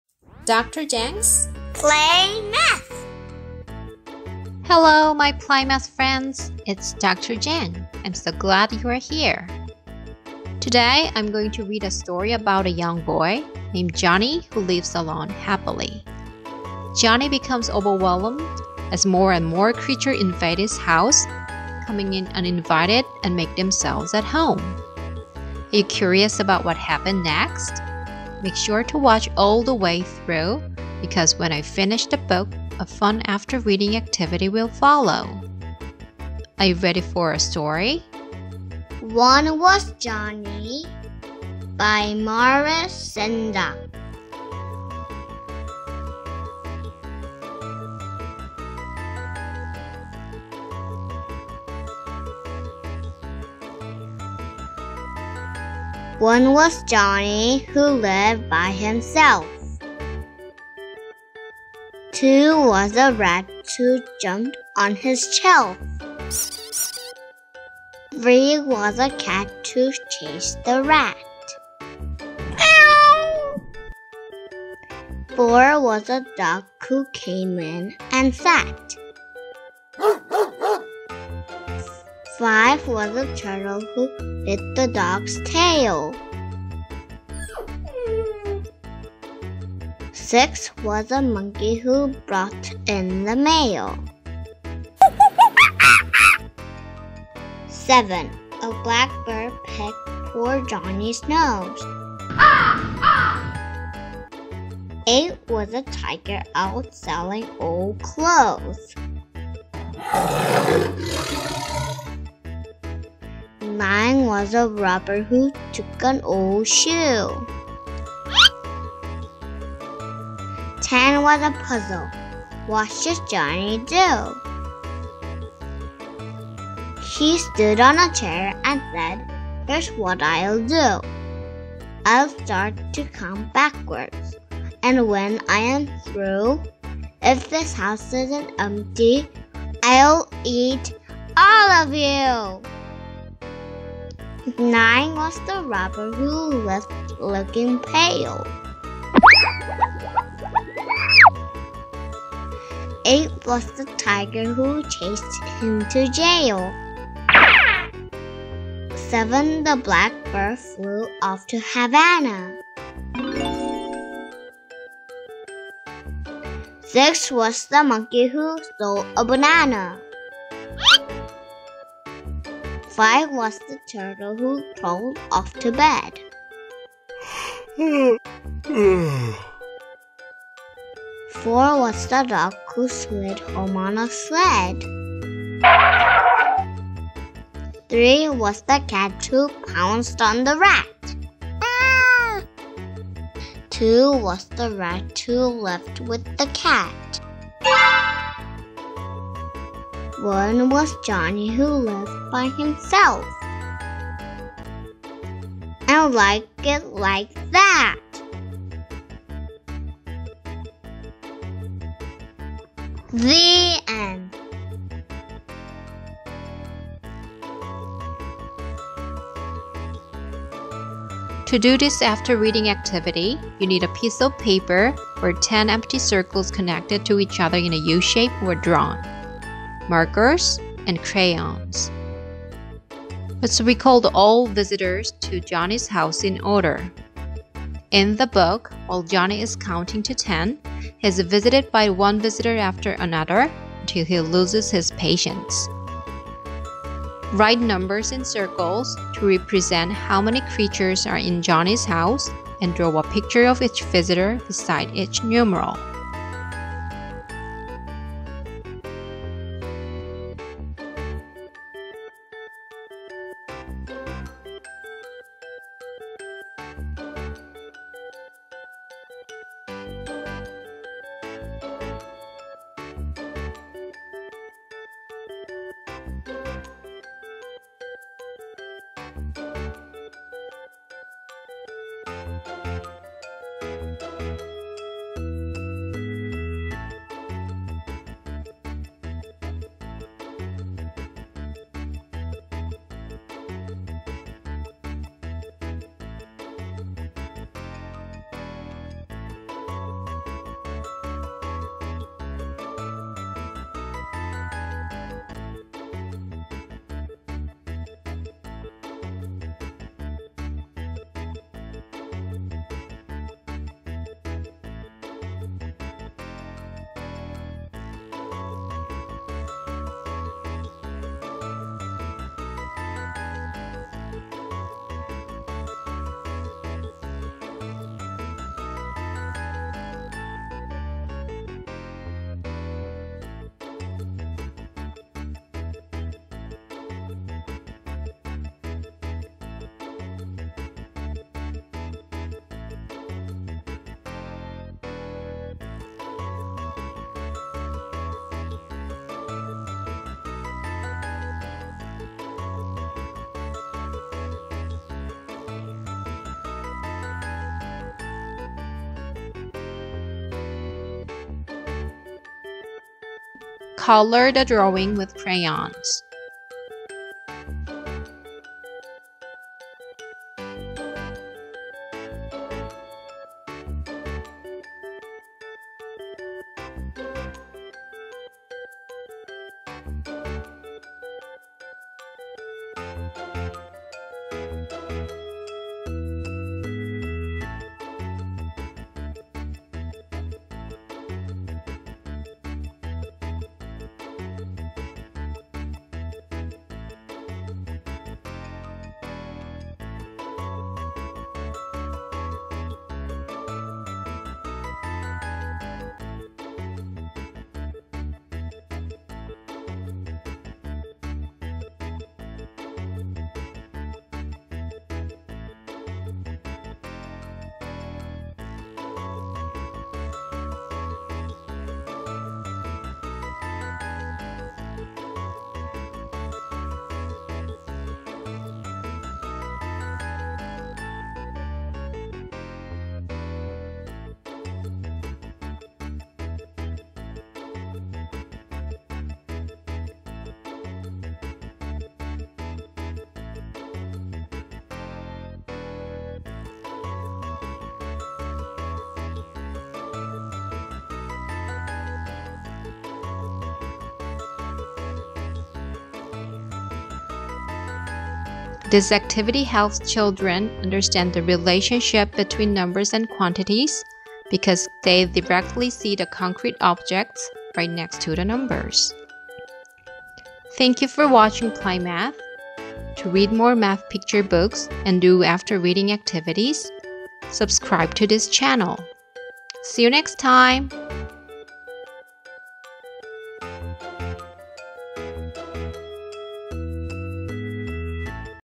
Kids Book Read Aloud | One Was Johnny by Maurice Sendak [Counting and Cardinality]
One Was Johnny
Written and illustrated by Maurice Sendak

Math skills developed by reading this book
1) Read Numerals from 1 to 10.
2) Understand the relationship between numbers and quantities.
3) Count backwards from 10 to 1.

You can find this book at Amazon